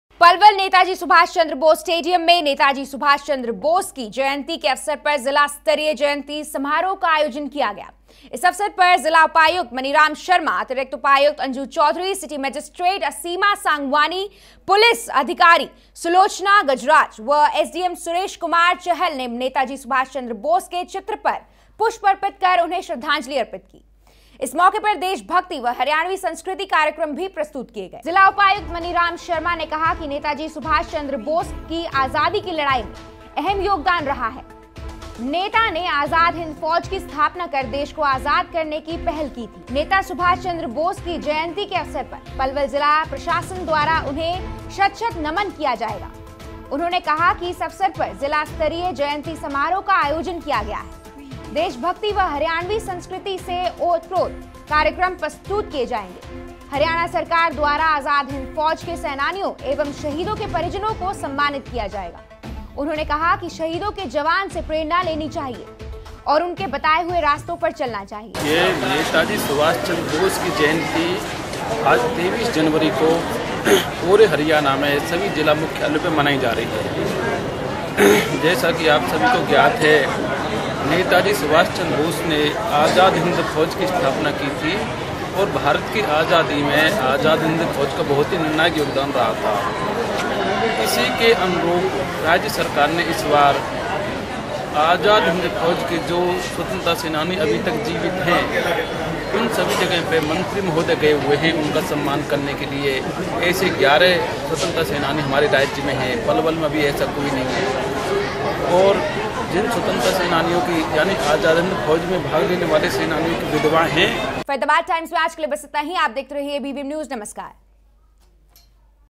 पलवल ║जिला स्तरीय जयन्ती समारोह का हुआ आयोजन
bvmnews haryananews topnews bvmnewslive latestnews पलवल, नेताजी सुभाष चंद्र बोस स्टेडियम पलवल में नेताजी सुभाष चंद्र बोस की जयन्ती के अवसर पर जिला स्तरीय जयन्ती समारोह का आयोजन किया गया। इस अवसर पर जिला उपायुक्त मनीराम शर्मा ,अतिरिक्त उपायुक्त अंजू चौधरी,सिटी मजिस्ट्रेट असीमा सांगवानी,पुलिस अधीक्षक सुलोचना गजराज व एसडीएम सुरेश कुमार चहल ने नेताजी सुभाष चंद्र बोस के चित्र पर पुष्प अर्पित कर उन्हें श्रद्धांजलि अर्पित की। इस मौके पर देश भक्ति व हरियाणवी सांस्कृतिक कार्यक्रम भी प्रस्तुत किए गए।